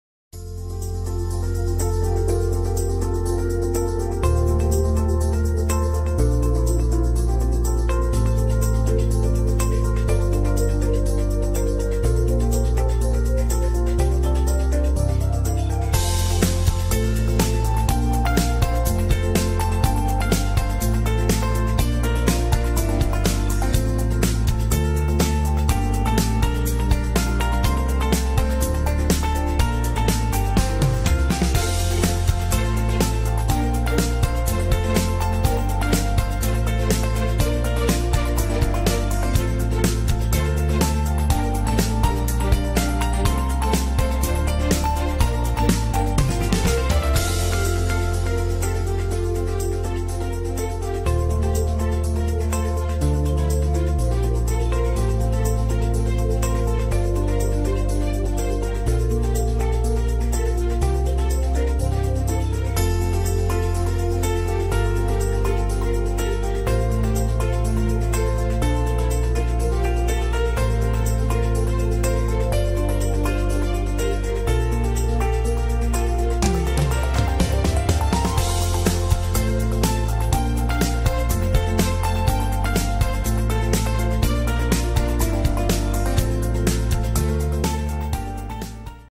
Wied's marmoset (Callithrix kuhlii)
Wied's marmoset

Wied's marmoset, also known as Wied's black-tufted-ear marmoset, is a New World monkey that lives in tropical and subtropical forests of eastern Brazil. Unlike other marmosets, Wied's marmoset lives in groups consisting of 4 or 5 females and 2 or 3 males. Wikipedia
Scientific name: Callithrix kuhlii

Rank: Species

** Wied's marmoset is highly social, spending much of its time grooming. It has individually distinctive calls, and it communicates through gestures and olfactory markings as well.

* Special thank you for Lumen5 - Video Maker (lumen5.com) *